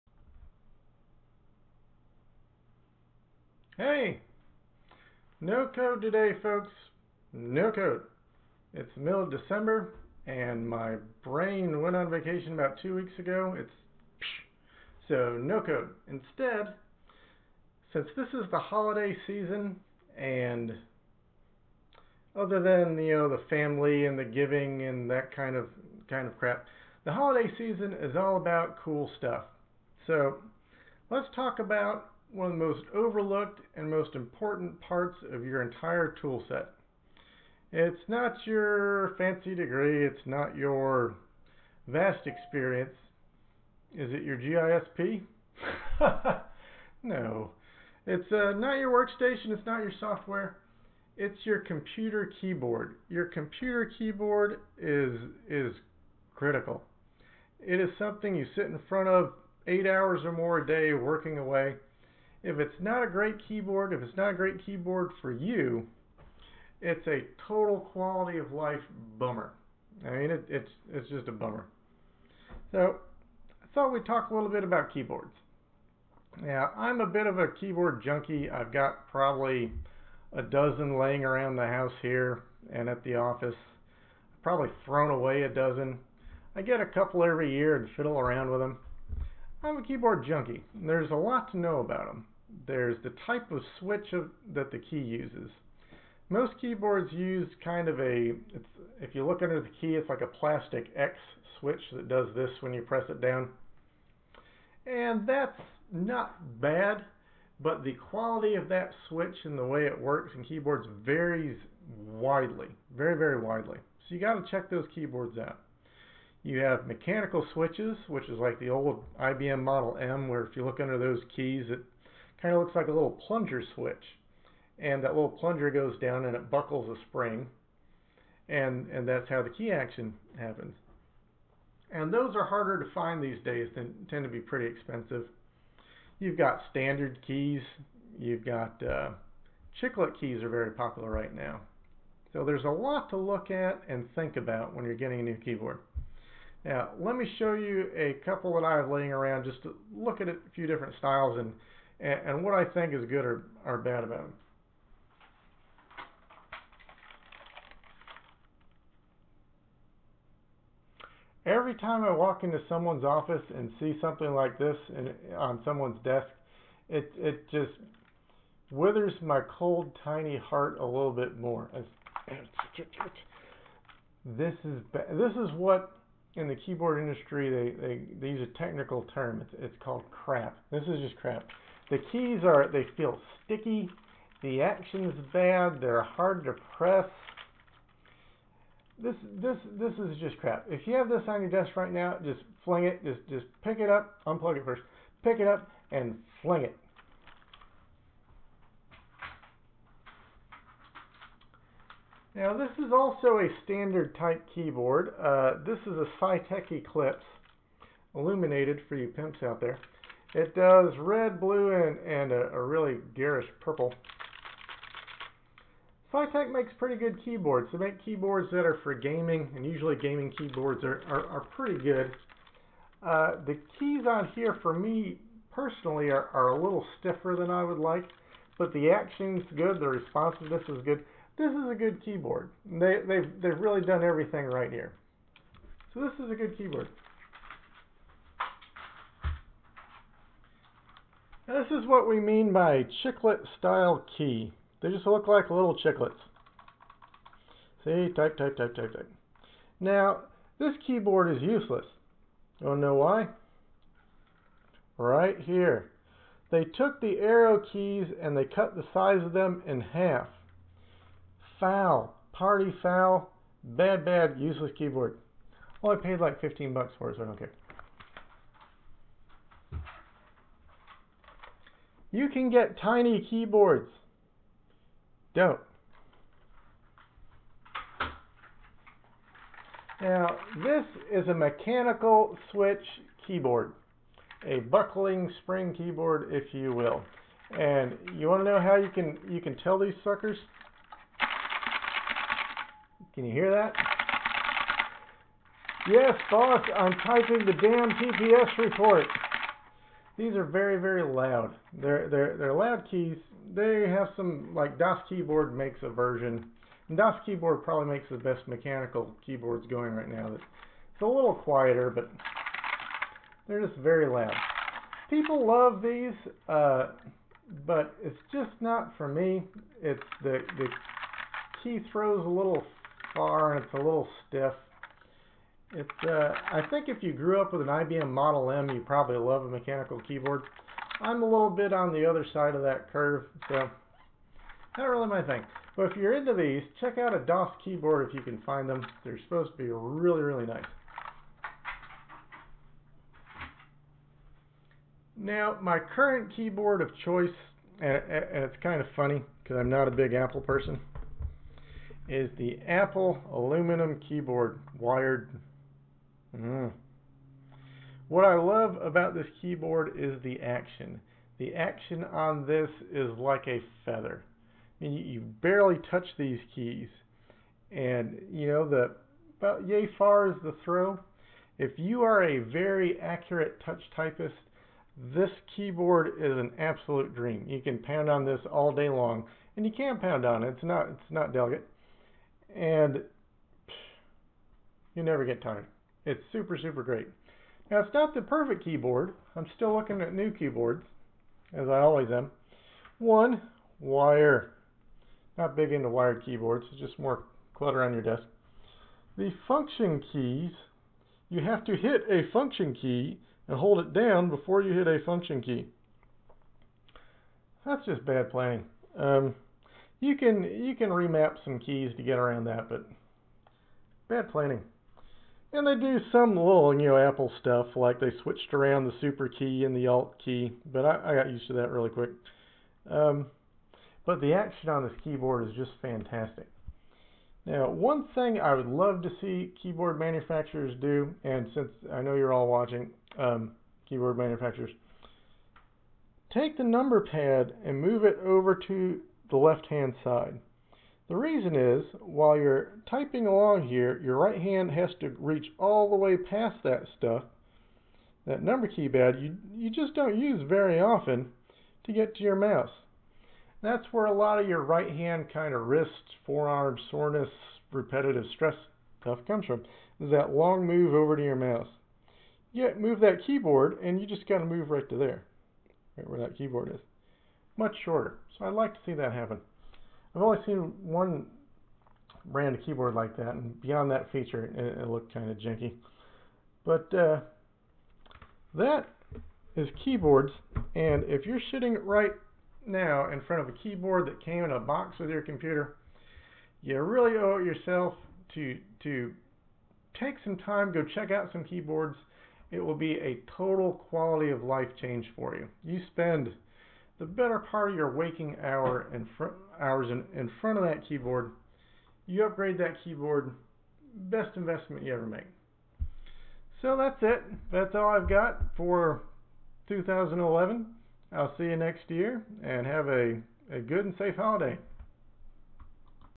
Keyboards
A review of different makes and models of computer keyboards.